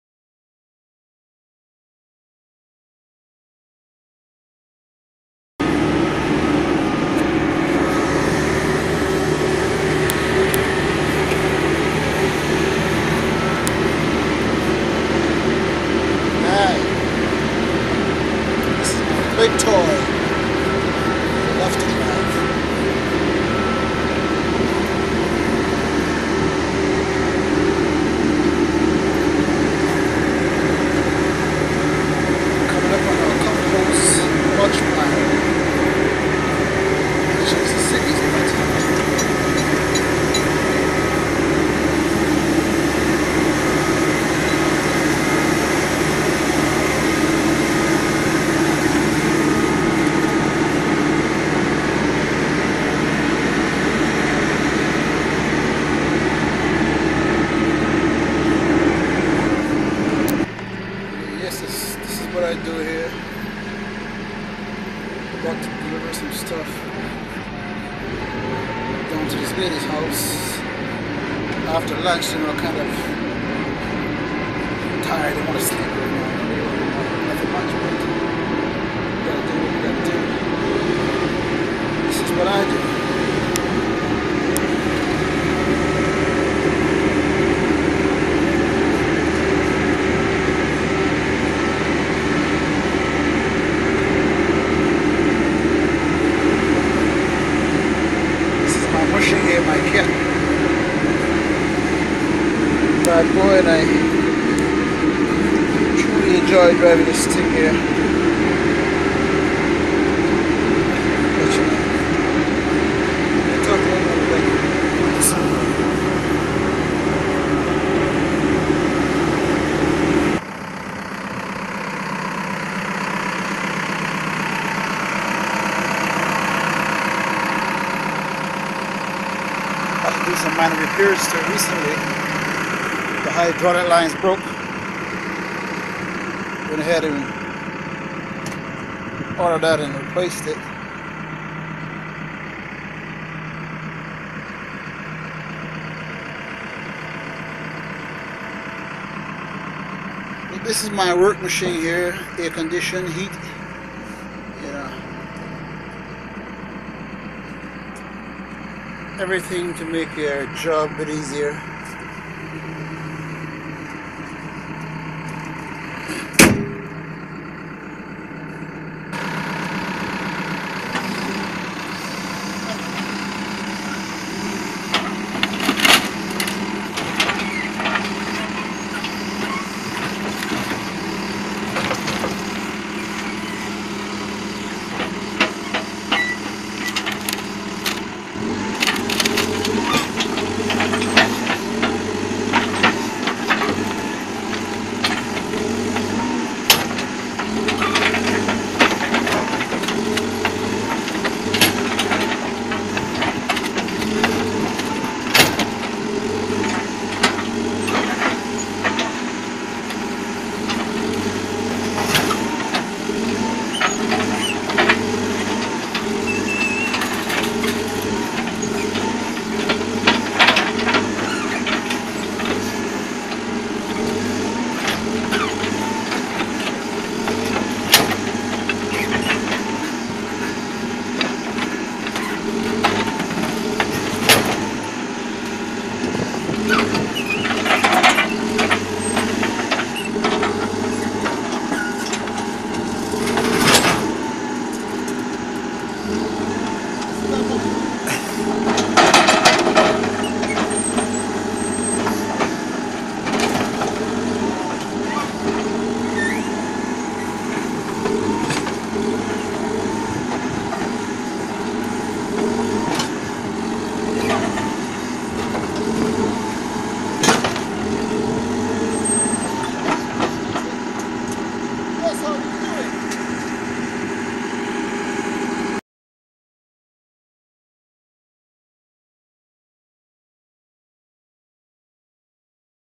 MY BIG CAT MACHINE!
A Black Man at Work on his Big Cat Machine